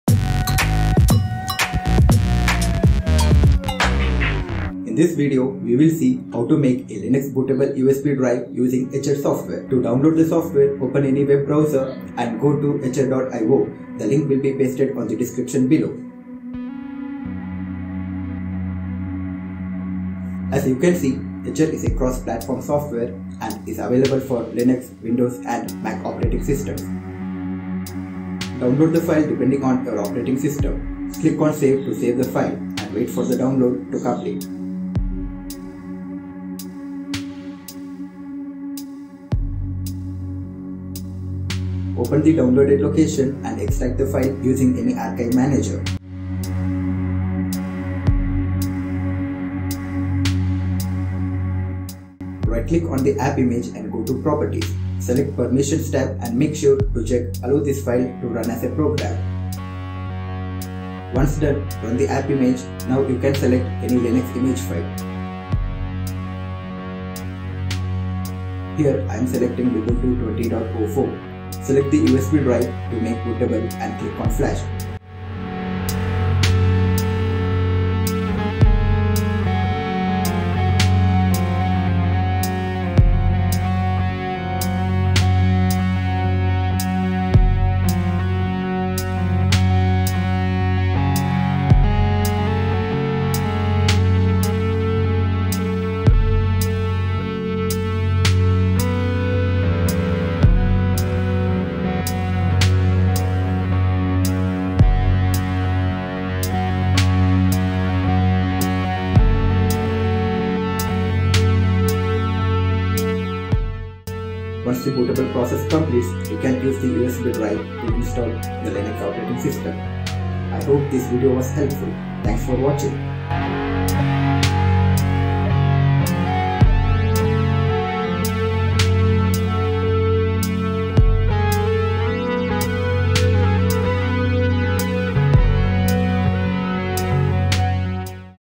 How to Make a Linux Bootable USB Drive Using balena Etcher
balenaEtcher developed by balena and is a free and open-source software (licensed under Apache License 2.0) used for writing image files such as .iso and .img files, as well as zipped folders onto storage media to create live SD cards and USB flash drives.